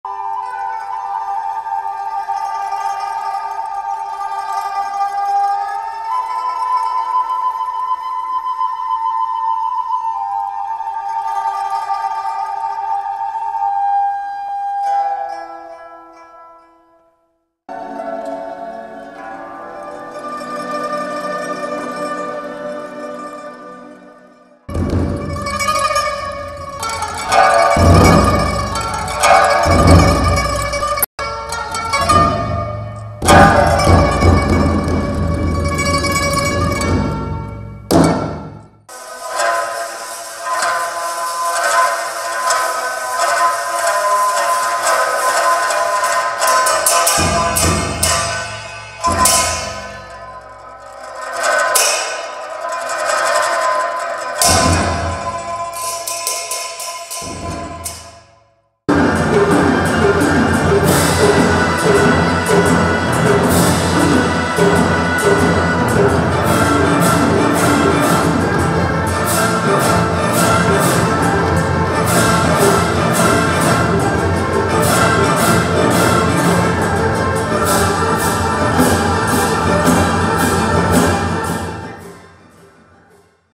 China National Traditional Orchestra kicks off NYC tour in Lincoln Center 中央民族乐团《泱泱国风》林肯中心上演
China National Traditional Orchestra began its New York tour Friday in Lincoln Center, showcasing the “Splendor of Folk Music”. The other concert “IMPRESSIONS: Rediscovering Chinese Music “is to be staged in Carnegie Hall on Sunday.